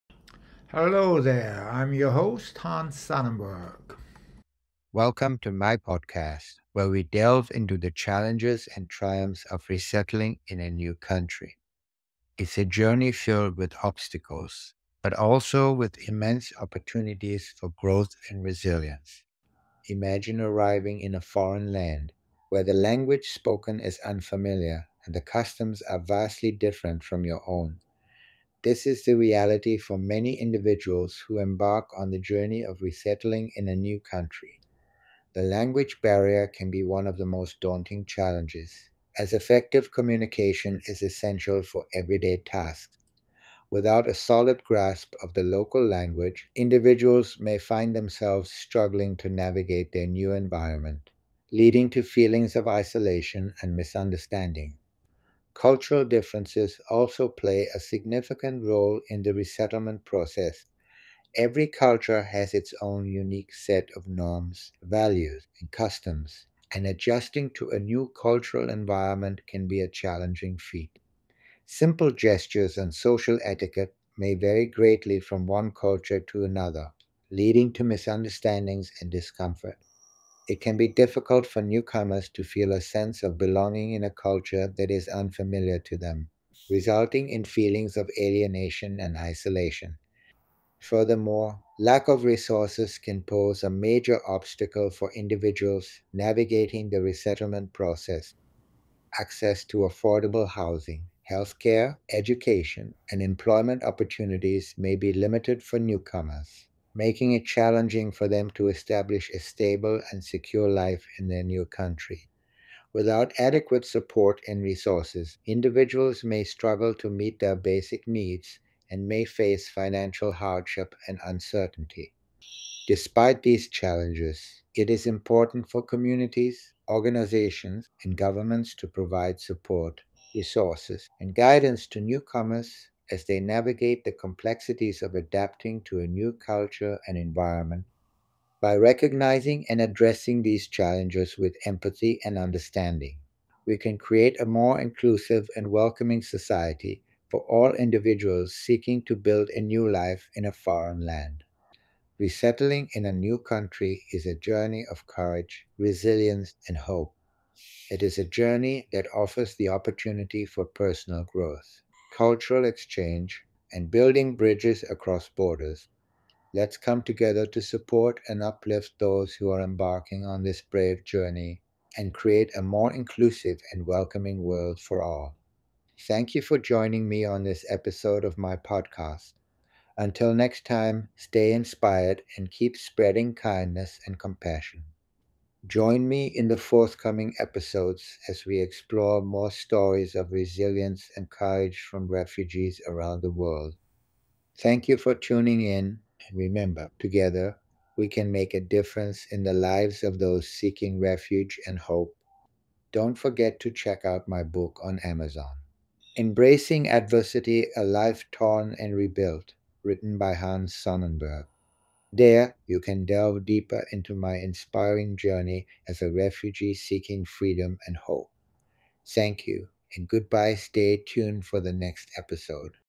Embracing Adversity Journey - Refugee Voices- Part 05
🎧 Dive into 'Embracing Adversity Journey - Refugee Voices- Part 05' where we uncover the real challenges faced by refugees resettling in a new country. From language barriers to cultural differences and the constant struggle with limited resources, this episode sheds light on the complexities of adapting to a new life and environment.

Join us as we explore the stories of resilience and courage that define the refugee experience. Listen to understand, and learn how we can all contribute to making their journeys a bit easier. Together, we can help bridge the gaps and build a more welcoming world. 🕊️

👉 Tune in now and be part of the conversation. Let’s support and stand with refugees as they navigate these immense challenges.